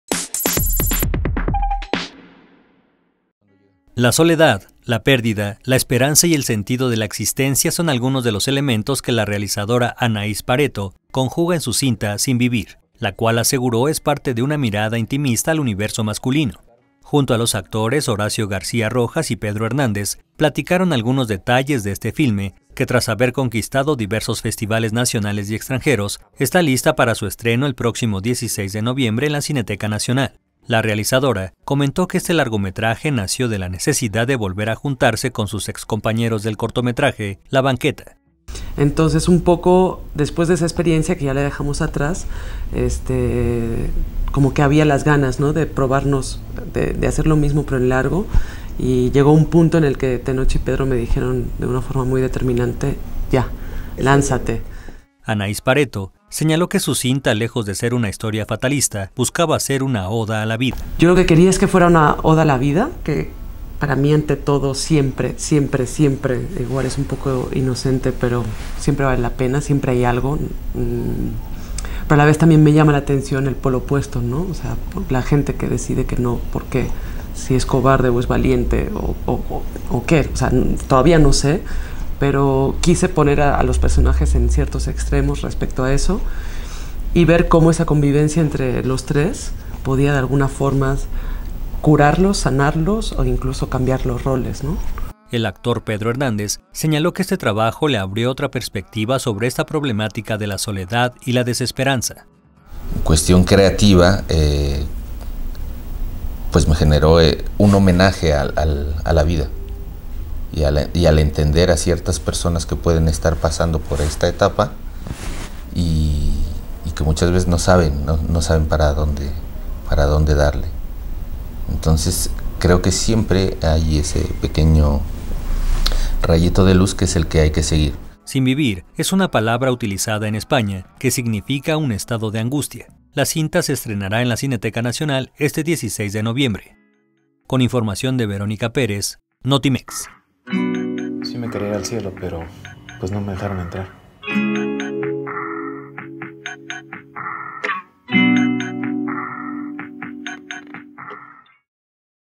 La soledad y la pérdida son exploradas en la cinta “Sinvivir”
La cinta estrenará en la Cineteca Nacional este 16 de noviembre